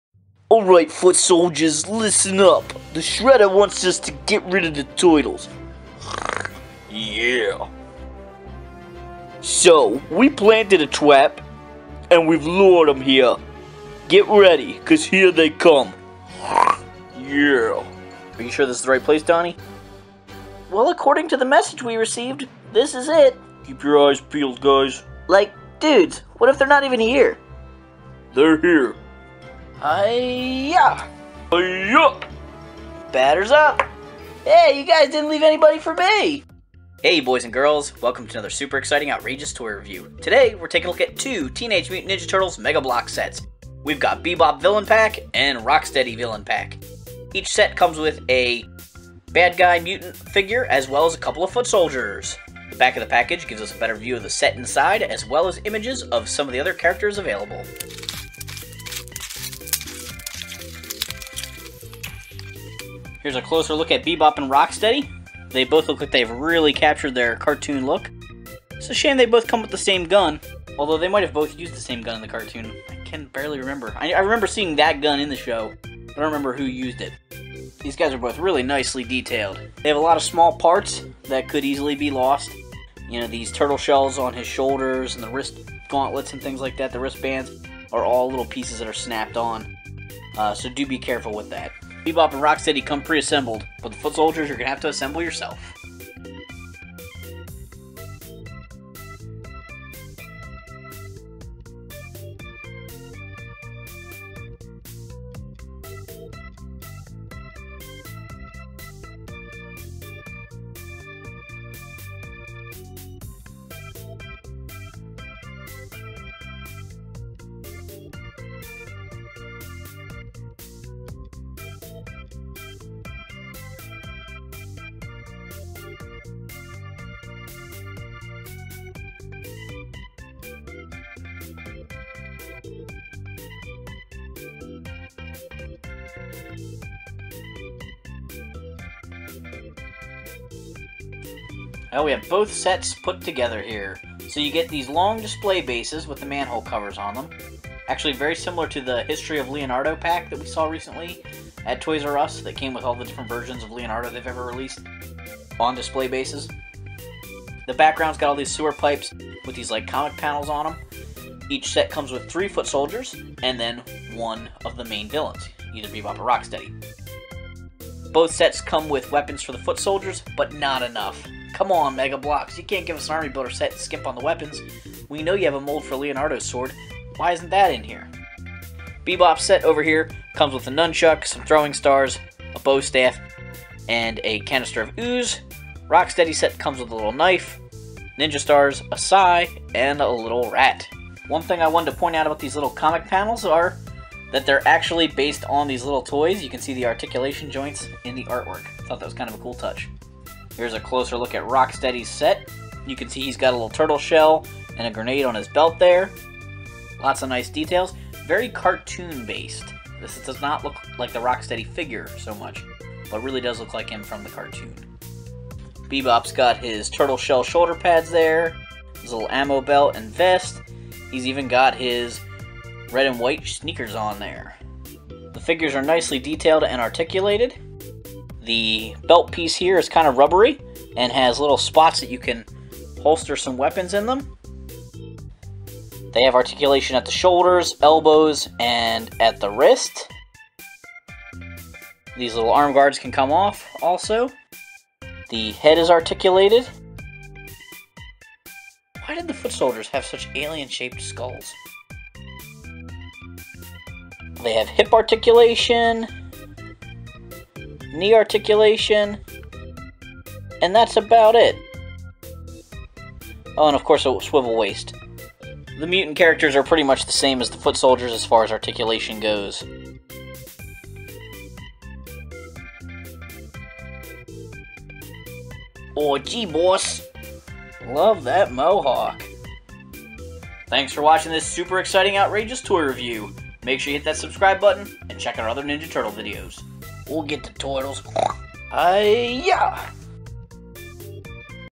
Bebop & Rocksteady Villain Sets w/ Foot Soldiers- Teenage Mutant Ninja Turtles - Mega Bloks
Bebop and Rocksteady Villain Packs from the Teenage Mutant Ninja Turtles Mega Bloks Collectors Line. Each pack includes a Mutant Character and three Foot Soldiers along with a display base.

About Us
Super Exciting Outrageous Toy Review reviews best-selling toys for girls and boys. Join us every weekday for more excellent toy reviews.